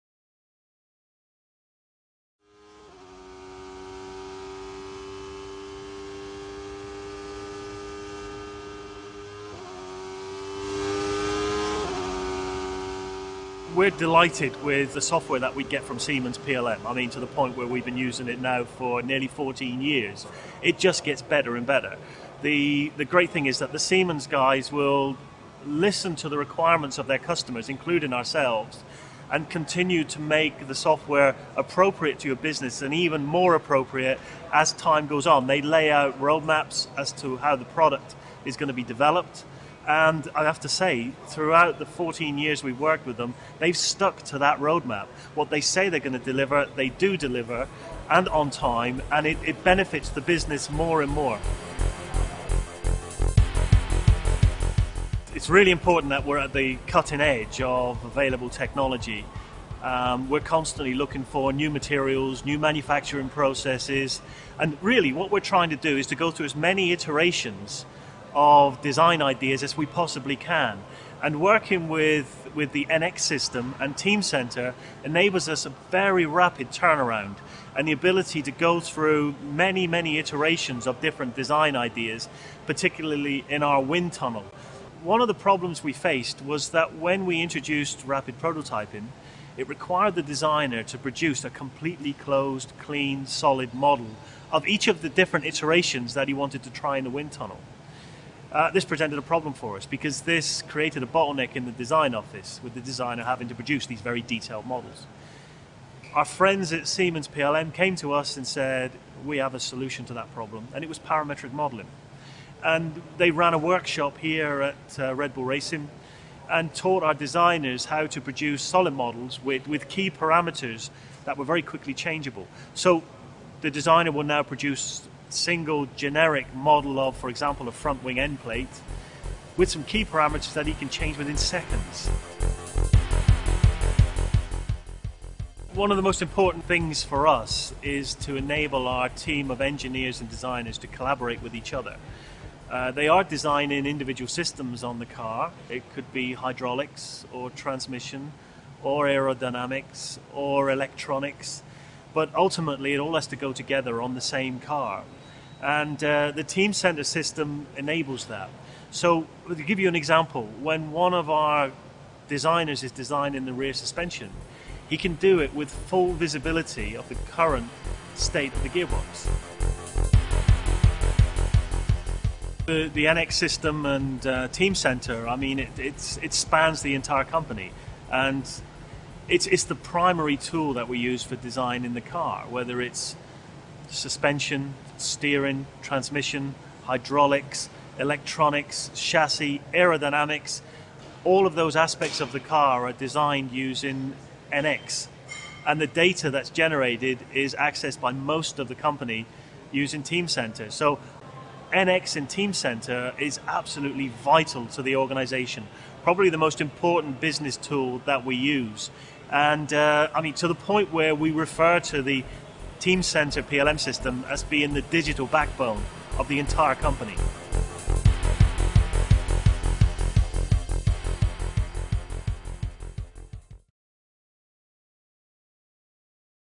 From virtual to real in one adrenalin rush Red Bull Racing - Siemens PLM Software
NX and Teamcenter are the digital backbone for a 600-strong team focused on strength, speed and spirit

Unrelenting focus on evolution and innovation
"There's no room for complacency in this sport," observes Christian Horner, team principal of Red Bull Racing, after the team finished second in the 2009 Championship. As he spoke, the team was already manufacturing its 2010 car, because Formula 1® success demands an unrelenting focus on evolution and innovation.

"We talk about racing 'cars,' but they cannot really be compared with what we drive on a normal road," comments Steve Nevey, Red Bull Racing's business and technical development manager. "The flow of air over, under and through a racing car determines its ultimate speed and the way it functions, for example the engine is fed and cooled by airflow. The emphasis on aerodynamics and weight means that we are designing and manufacturing technical machines that are closer to an aircraft than a saloon (sedan) car. They are finely balanced to begin with and then continually refined for speed and accuracy."

Red Bull Racing's development process has to be as swift and precise as these amazing vehicles and the company rely on NX™ software for CAD/CAM/CAE and Teamcenter® software to capture and manage knowledge. It is a creative and collaborative, virtual world from which the real, live car emerges almost fully formed and ready to be put to the test on the track. "We call it our digital backbone," says Nevey. "NX and Teamcenter sit at the heart of the company and they feed data to our manufacturing facilities; we could not manage without them."